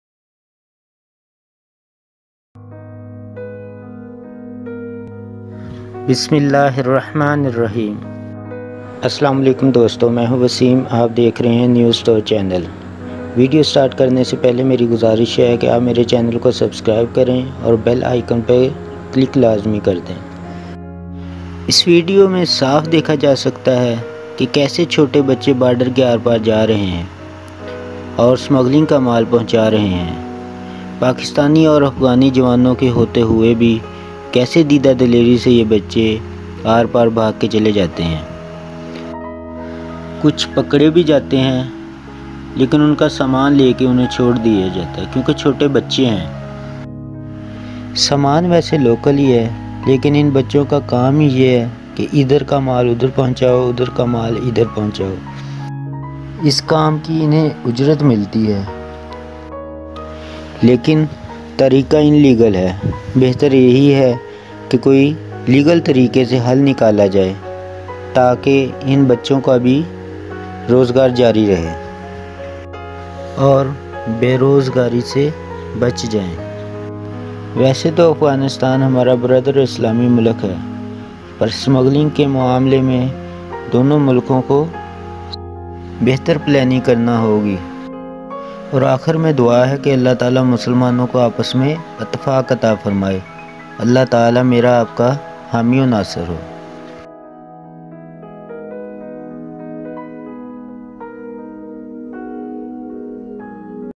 CCTV Footage | Bachon Ki Dida Dlairi Dekhen Smuggling Kar Rahe Wo B Jawano Ke Samne... {News Store}
Es Video Ko Like or Share Kar den Please.